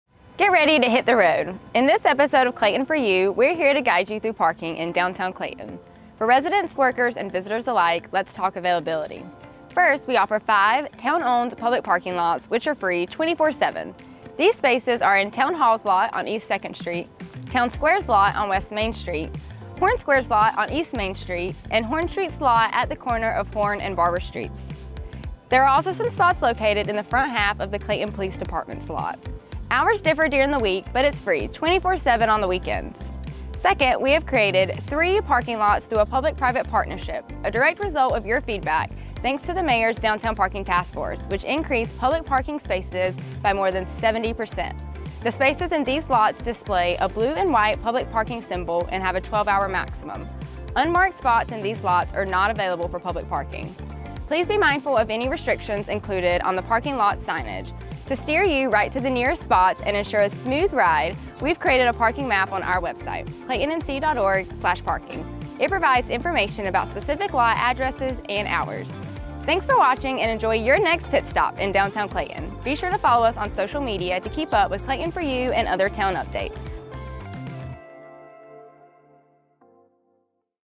Clayton4U - Downtown Parking
Buckle up and get ready to hit the road! This episode of Clayton4U is all about navigating the parking scene in Downtown Clayton.